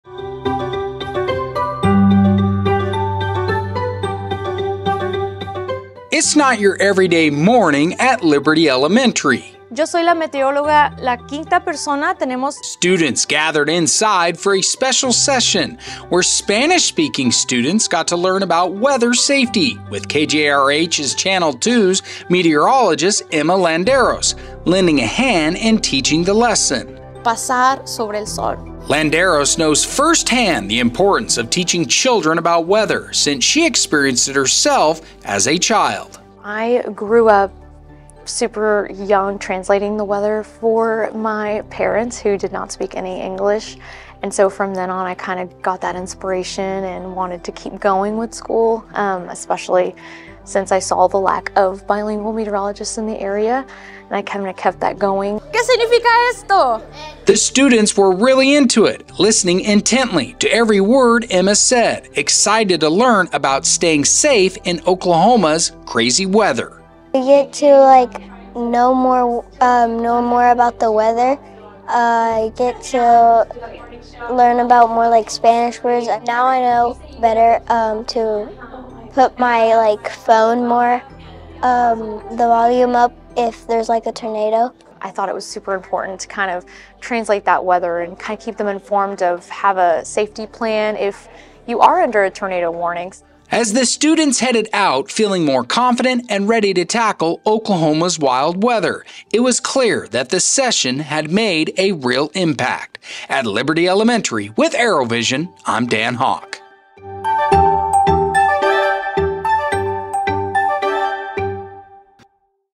Empowering Safety: Liberty Elementary's Morning with Meteorologist Emma Landeros
Experience Liberty Elementary's unique morning session with Meteorologist Emma Landeros, where students learn vital weather safety tips. Emma's passion and personal experience drive an engaging lesson, leaving students empowered to navigate Oklahoma's unpredictable weather.